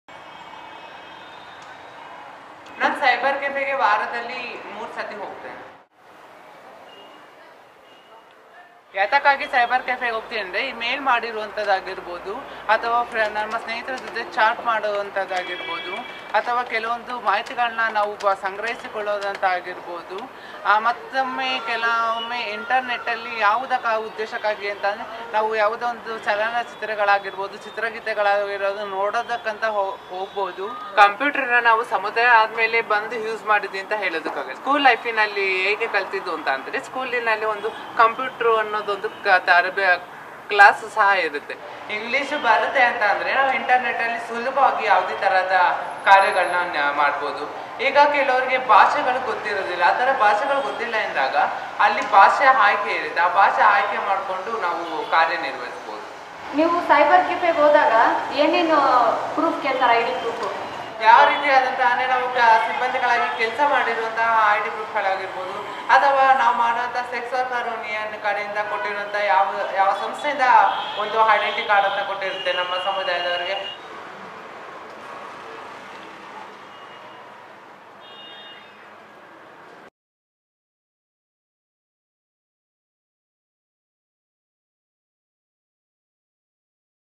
Transgender Interaction with Cyber Cafes
How does a transgender person access the Internet? Christy Raj in Bangalore trails a transgender member of his community as she visits the cyber cafe, interviewing her about language limitation, ID proof and what she uses the net for.